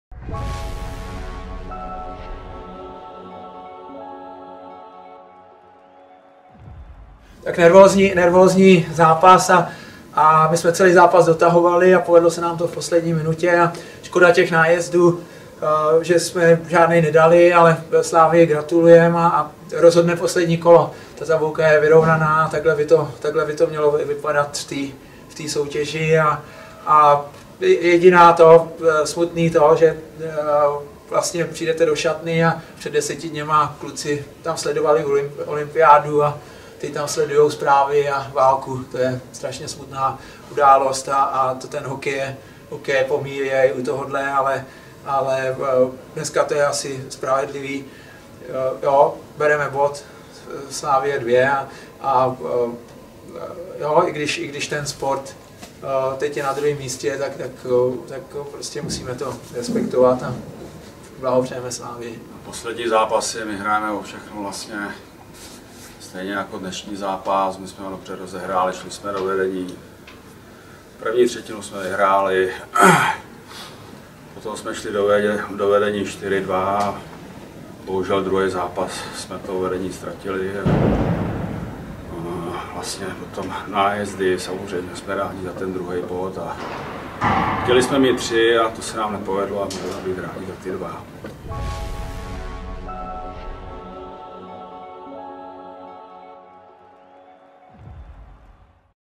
Tisková konference po utkání s Prostějovem (Pavel Kolařík, Aleš Totter)
Tisková konference po utkání 50. kola Chance ligy, ve kterém sešívaní vyhráli 5:4 po nájezdech nad Prostějovem.